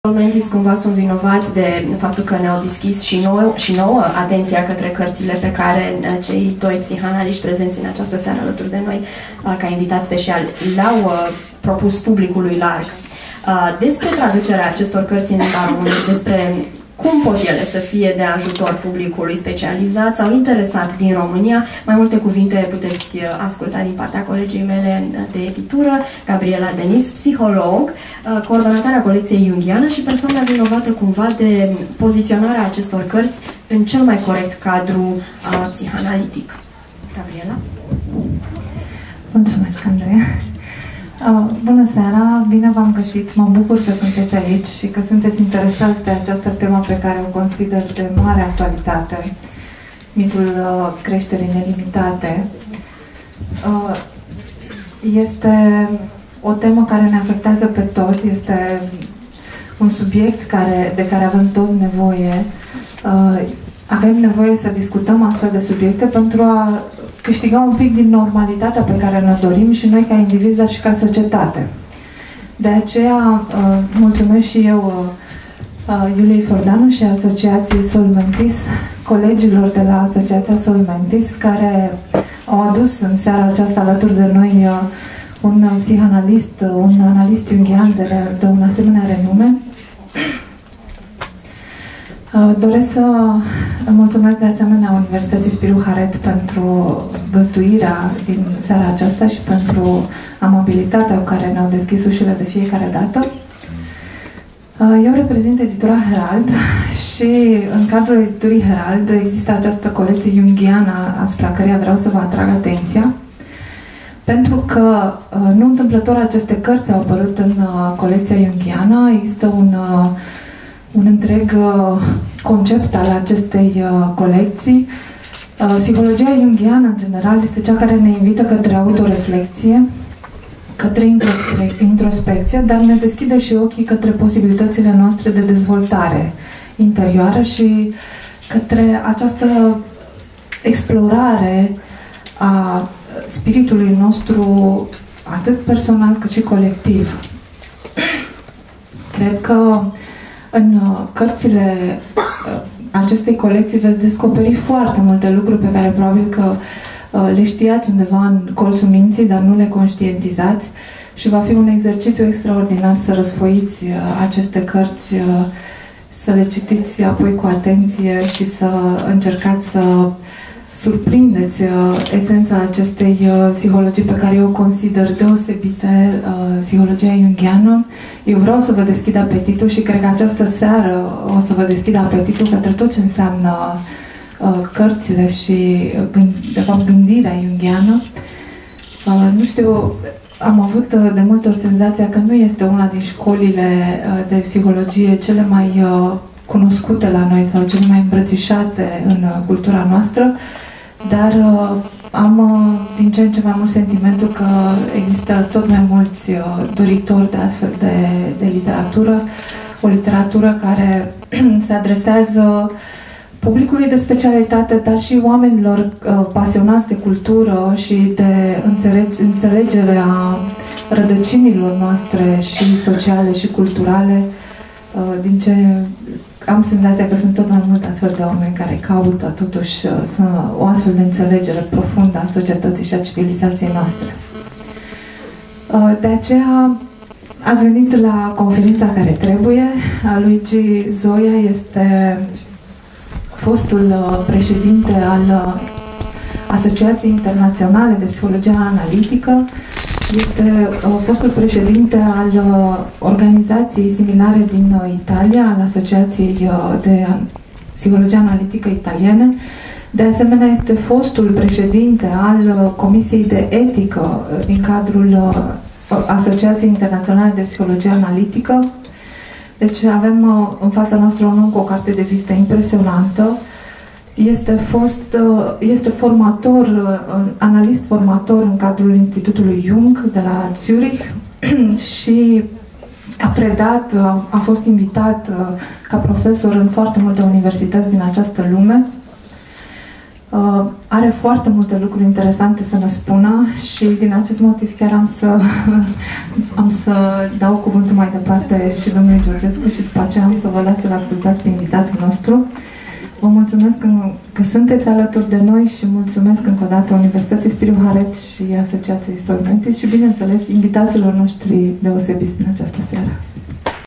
Gabriela Deniz, psiholog, coordonatoare colectie Jungiana, ed. Herald - Lansare "Istoria arogantei"
Joi 12 iunie, de la orele 19.00, la Sala Studio a Universităţii Spiru Haret a avut loc lansarea unui volum ce cartografiază dezvoltarea civilizaţiei europene: "Istoria aroganței", de Luigi Zoja. Lansarea pe piaţă a cărţii a beneficiat de prezenţa la Bucureşti a autorului, care a susţinut conferinţa publică intitulată "Mitul creşterii nelimitate. O psihologie a aroganţei şi pedepsei."

Tematica acestei conferinţe se regăseşte pe larg în cartea "Istoria aroganţei. Psihologia şi limitele dezvoltării", publicată de editura Herald, în colecţia JUNGIANA. Evenimentul are ca public țintă specialiştii din domeniul psihologiei, psihoterapiei, psihanalizei şi antropologiei, dar şi publicul larg interesat de marile teme psihologice şi economice ale epocii noastre!

Au participat:
- Luigi Zoja, analist jungian şi scriitor italian;
- Prof. Laura Goran, specialist psihologia educației;
- Matei Georgescu, psihanalist;
- Iulia Feordeanu, psiholog, preşedinta a Asociaţiei Sol Mentis;
- Gabriela Deniz, psiholog, coordonatoarea colectiei Jungiana, ed. Herald;
- Andreea Tincea, PR Executive, ed. Herald.

Mai jos, înregistrarea video + audio + fotografii de la eveniment.